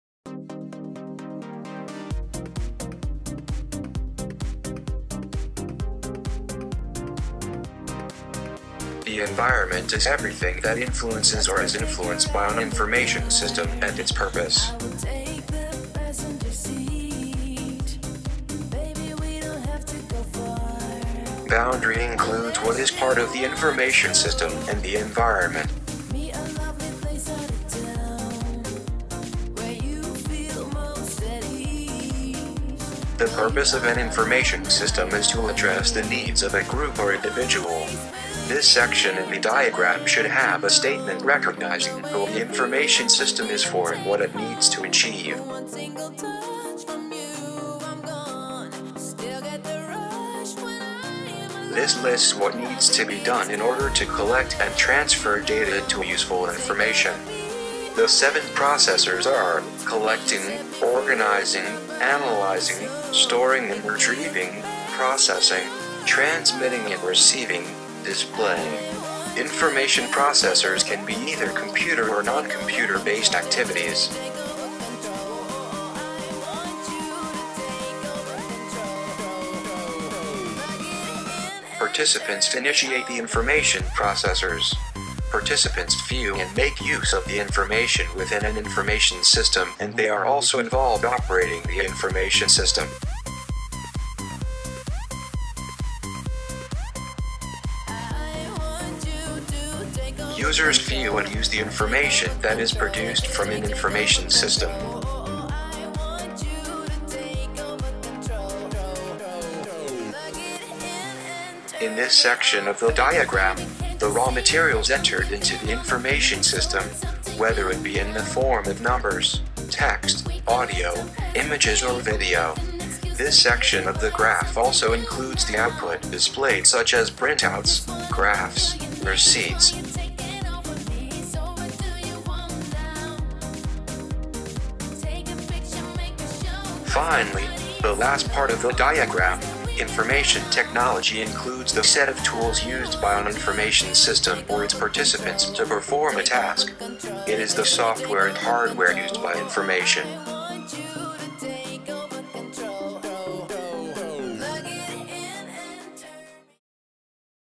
Diagram Representation of an Information System
This video is educating viewers about all the elements that make up an information system diagram. Hope you learn something :)